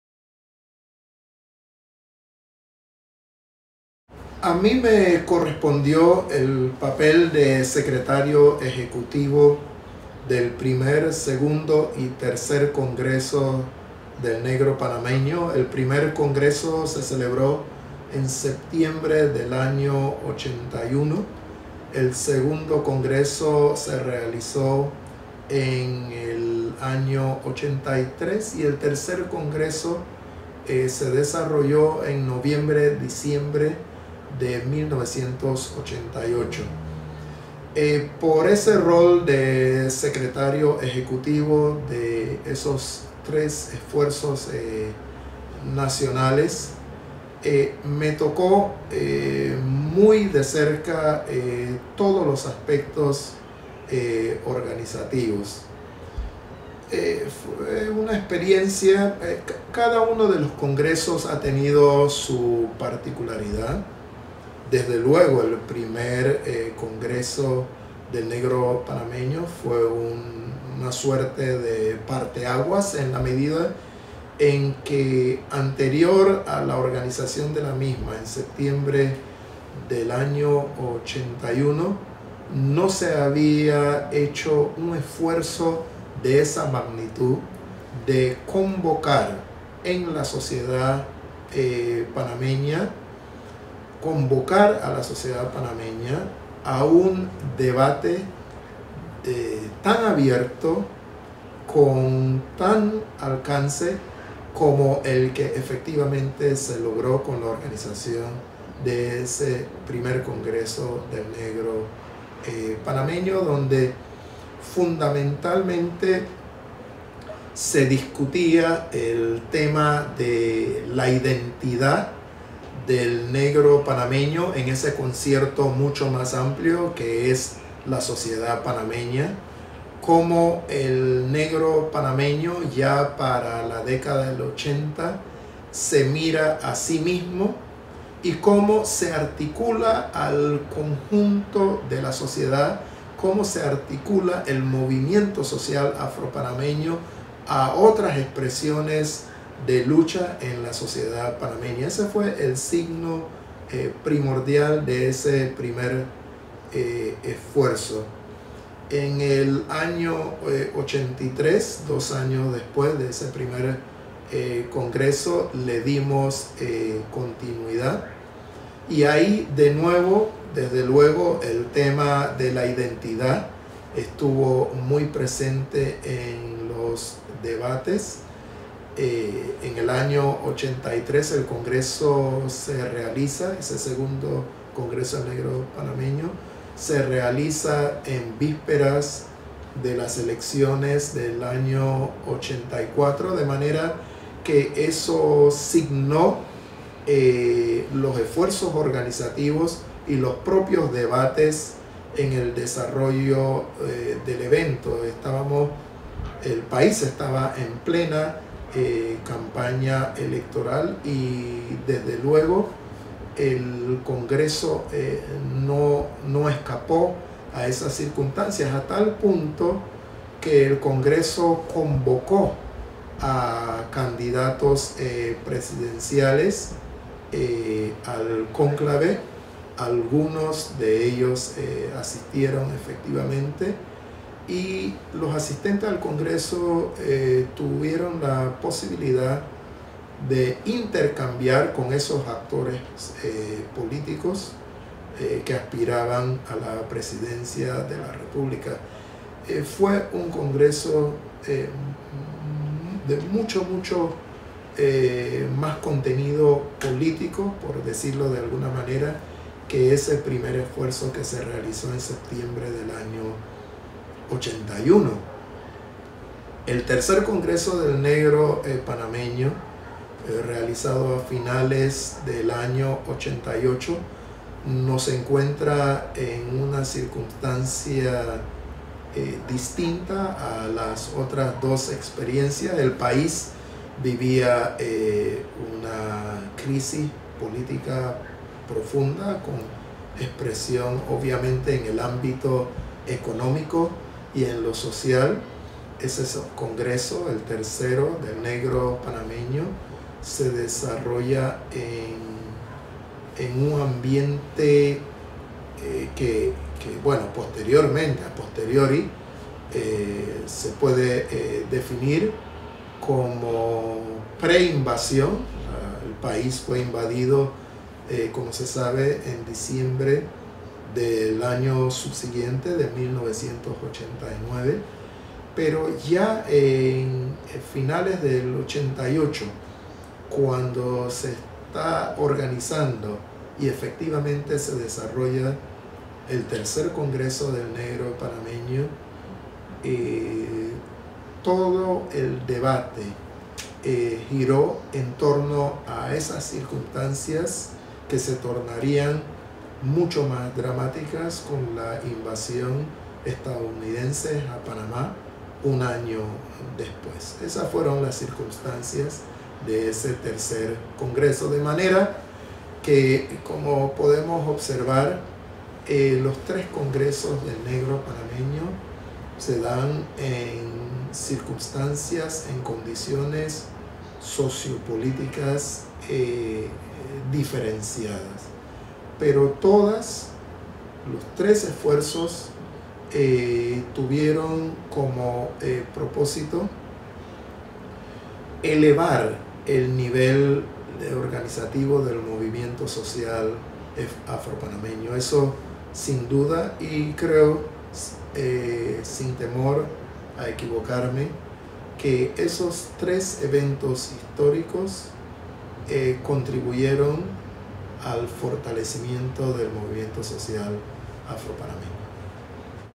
Proyecto Afrolatin@ presenta a Conversemos Afro con Alberto Barrow (Congresos Del Negro Panameño)
Lic. Alberto Barrow hablando sobe los Congresos de Negro Panameños de su punto de vista como  secretario ejecutivo de los Congresos. El primer Congreso (1981) 2do Congreso (1983), 3er Congreso (1988).
Alberto Barrow, Eq. discussing his role in the Congresses of Black Panamanians speaking from his experience as the Executive Secretary of the Congresses. The 1st Congress was in 1981, the 2nd in 1983, and the 3rd in 1988.